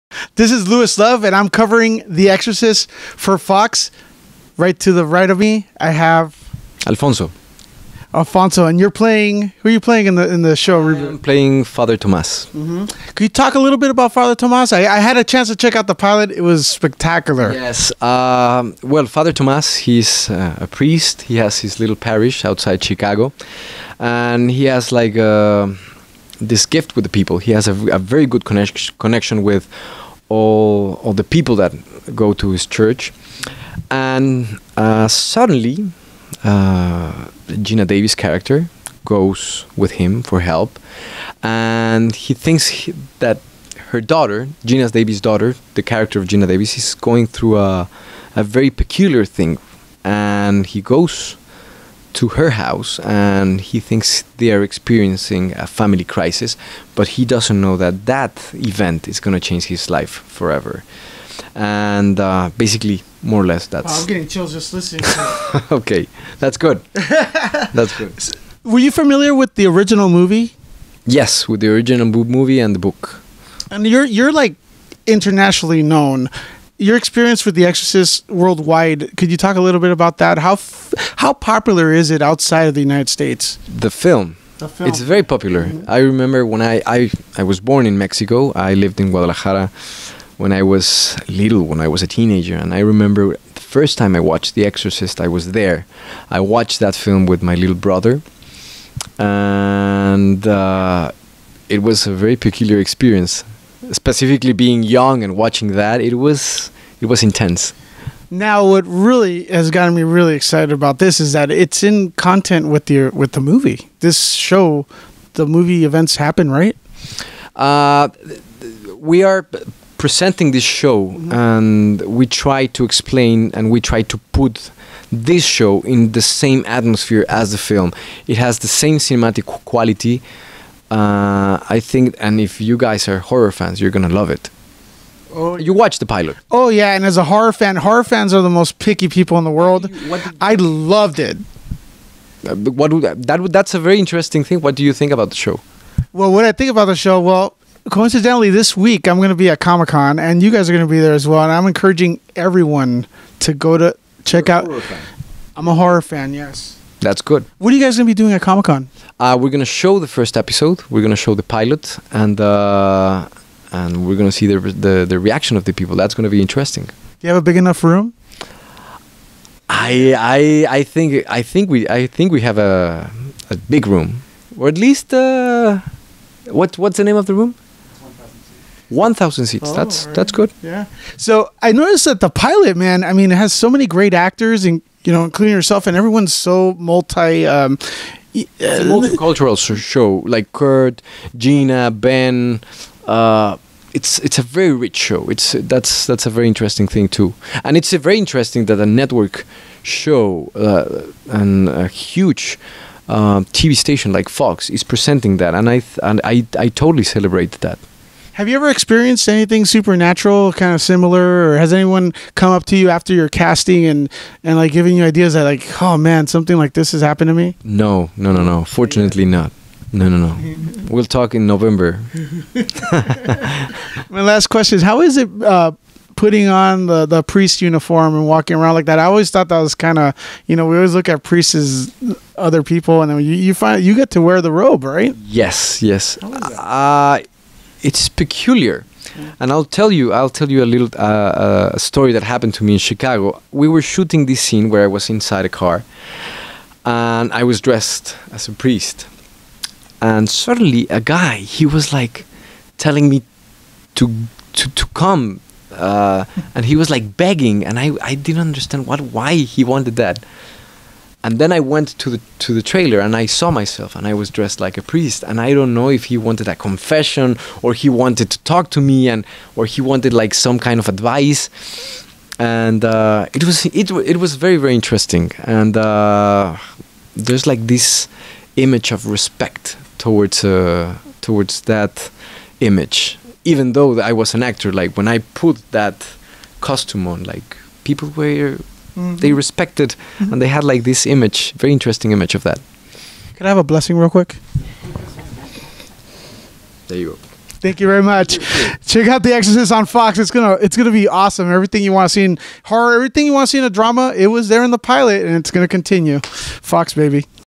Interview with Alfonso Herrera about FOX's THE EXORCIST
TV show based on William Blatty's 1971 novel about a priest who performs exorcisms on demonic spirits.